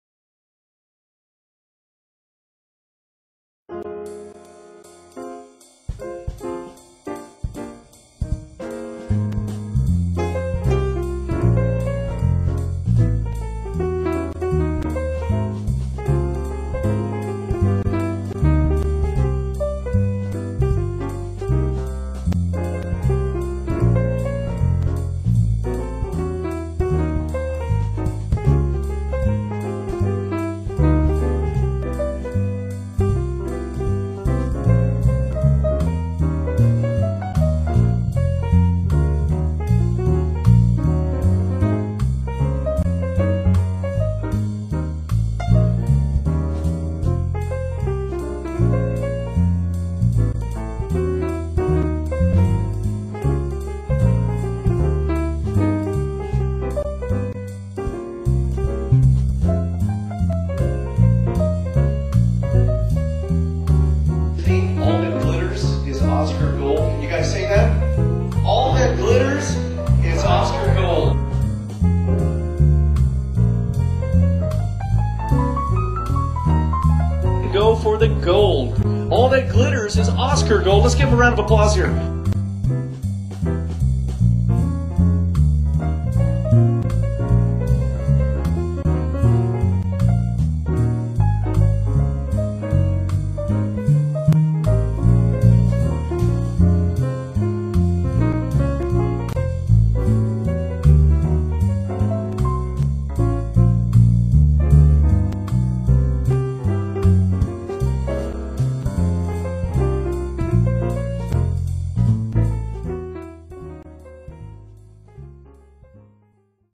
Oscars_2011.wmv
Highlight video from the 2011 A Night at the Oscars fundraising event.  The evening is hosted by the Business Division at LCSC and all procedes from the evening fund student scholarships.